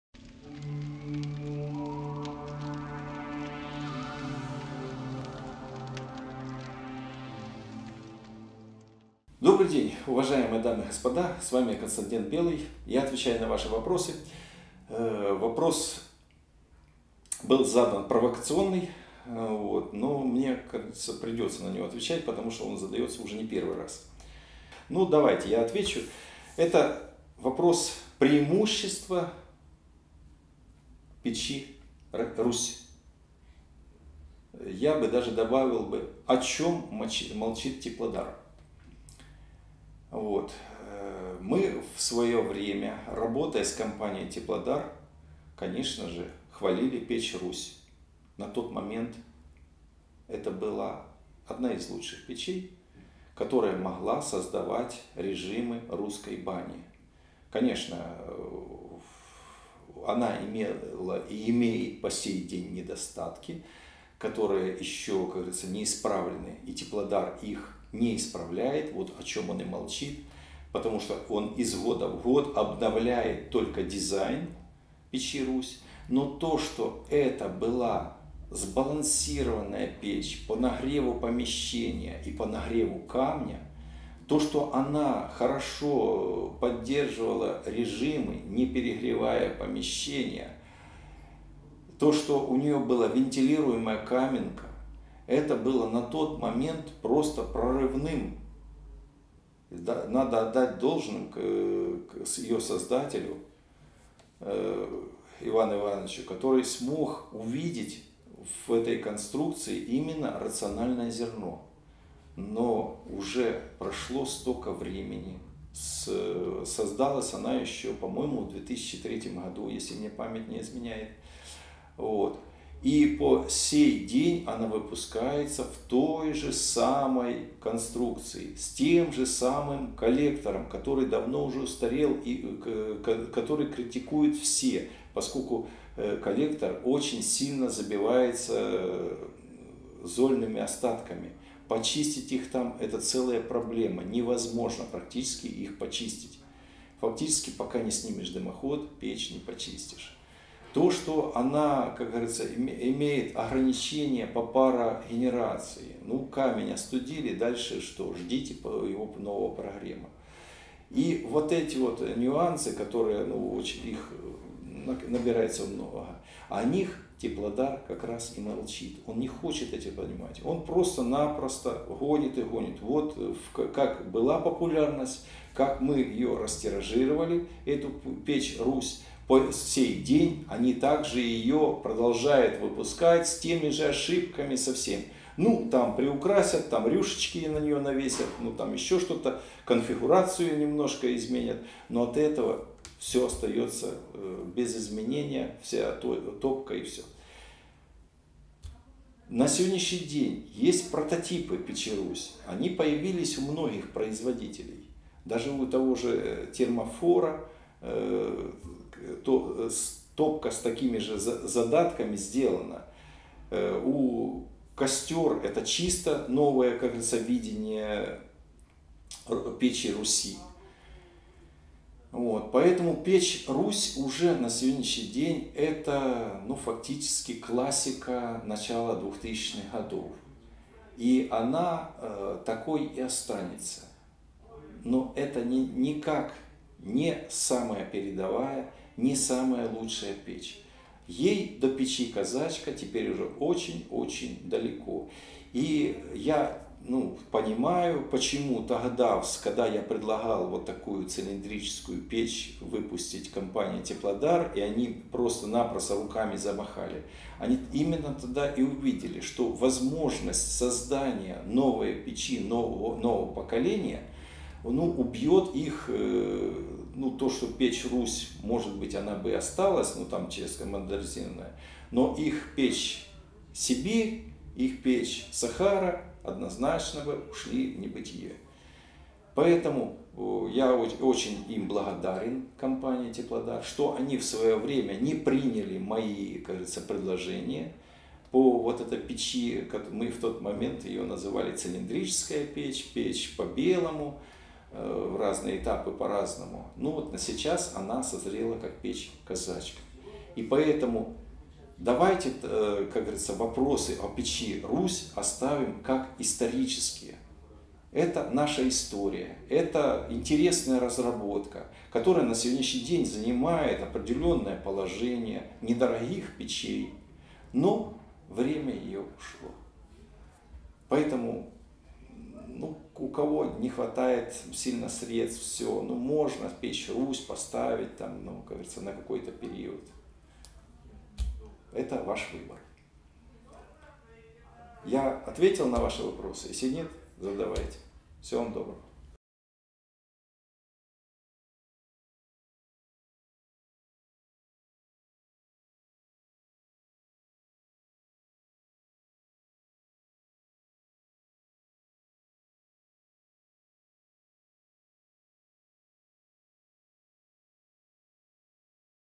Преимущества печи Русь О чем молчит Теплодар
Печь, если она имеет нарекания и она не совершенствуется - значит производитель не знает что с ней делать